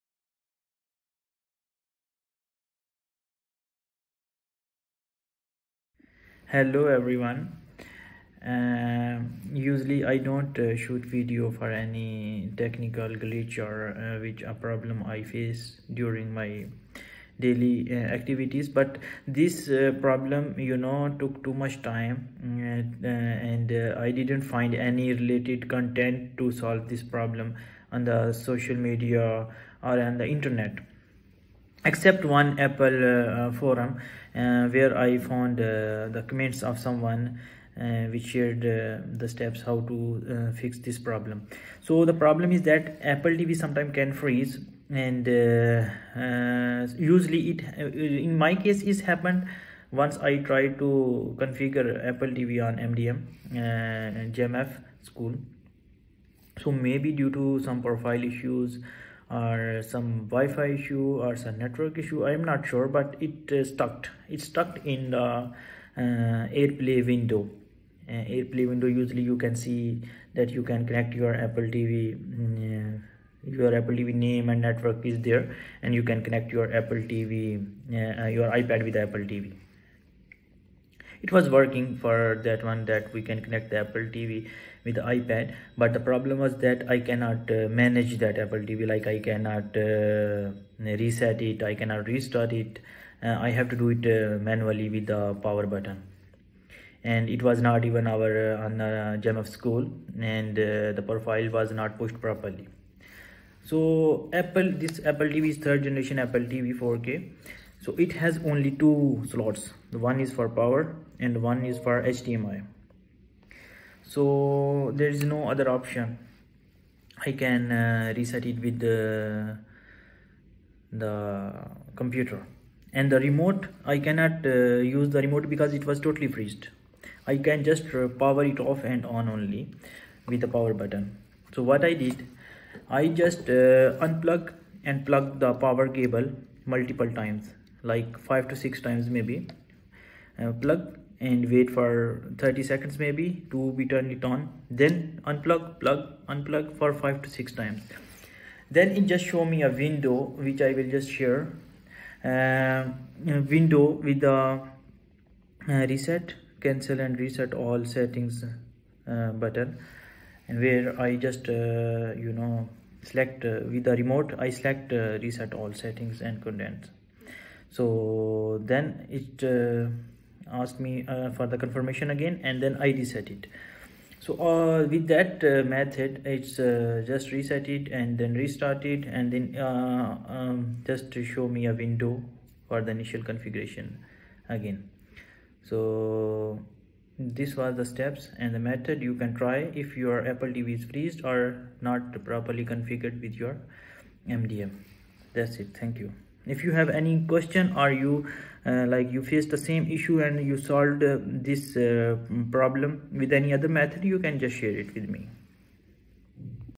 Apple TV Reset Guide
I recently encountered an issue with an Apple TV that was stuck on the AirPlay window and not showing up in Jamf School for management.
I was not able to reset it with Remote even.

After some troubleshooting, I found a simple fix that worked:
✅ Unplug and plug in the power cable 5-6 times
✅ Wait for the screen with Reset, Restart, and Restore options
✅ Select Reset All Settings and proceed
✅ Once reset, reconfigure the device successfully

I documented the process in a video to help others facing similar issues. Hope this helps!
If you have encountered same issue and did with any other method. Please comment or share it with me.